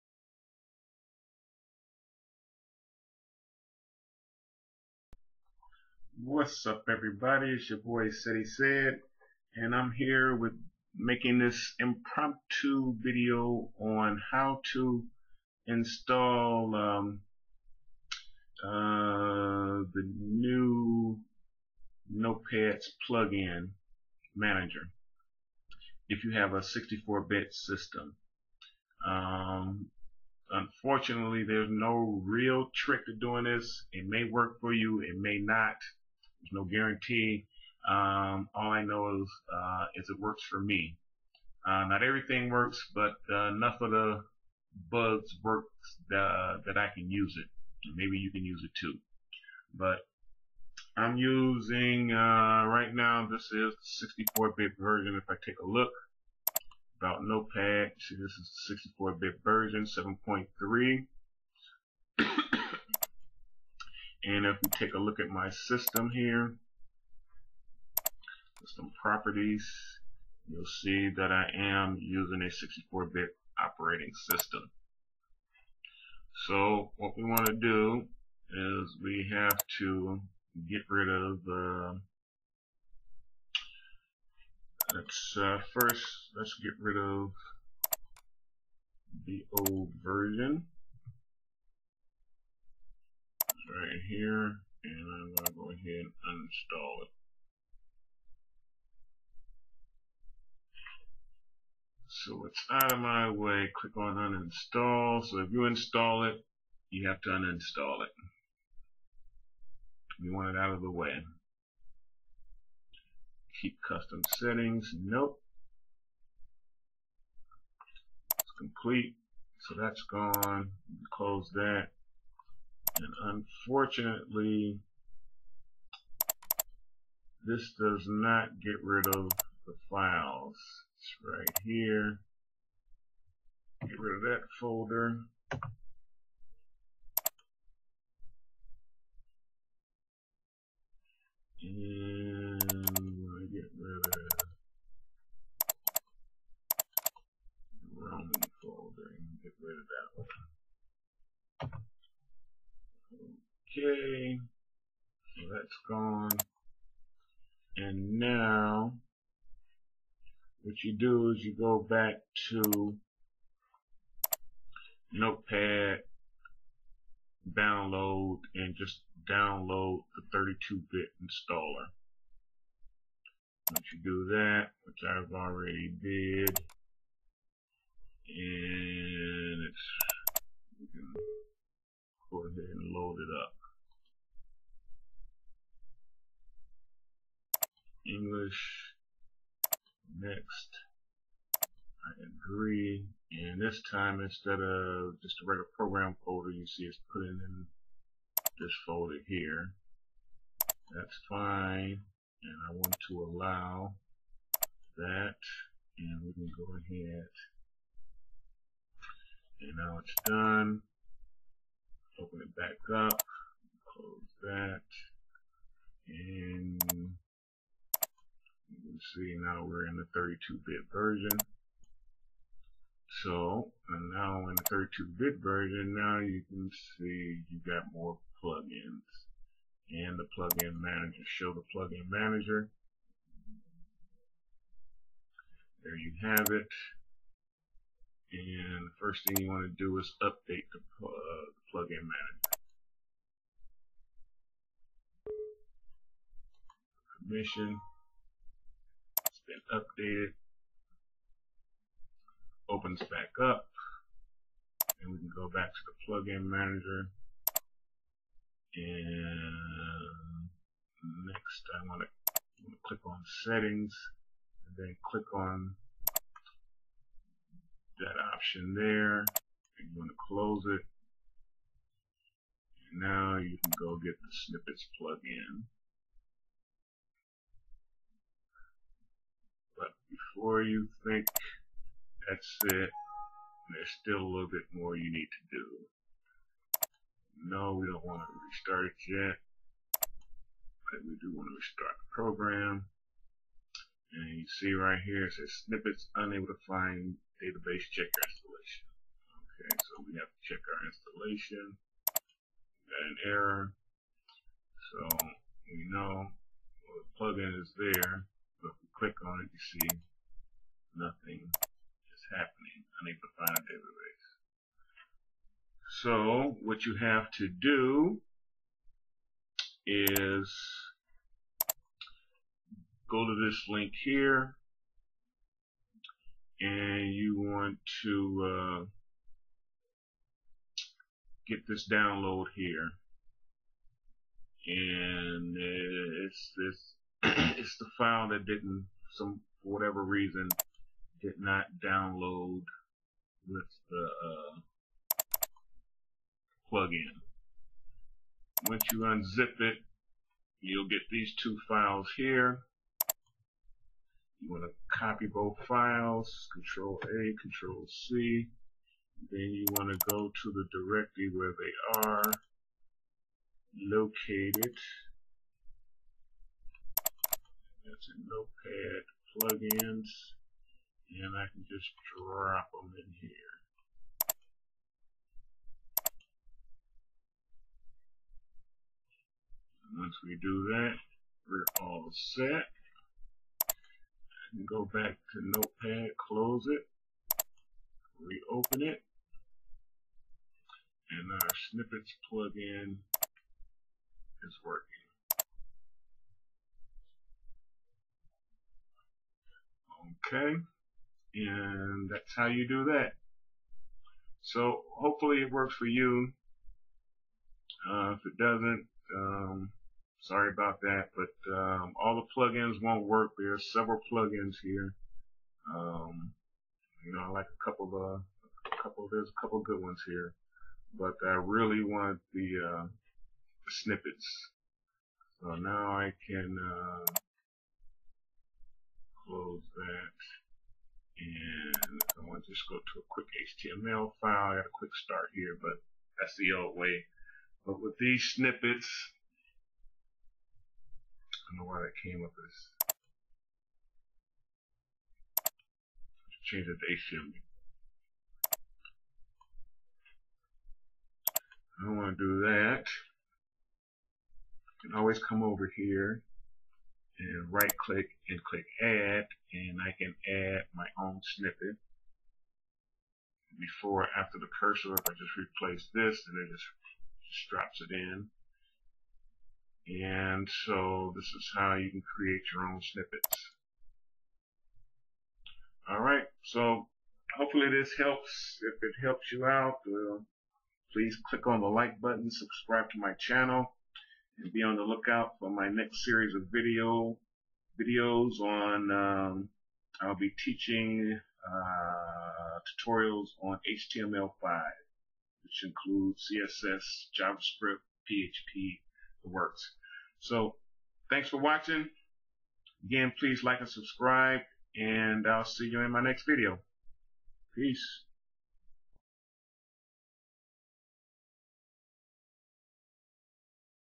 How to install Notepad++ Plugin Manager and Snippets plugin on 64 bit operating system
In this video, I show you how to install the Notepad++ Plugin Manager on a Windows 64-bit operating system. I also show you how to install the very cool notepad++ snippets plugin which can be a little tricky.

Even though all the plugins don't work, those that do will keep you going until the 64-bit plugin manager is ready.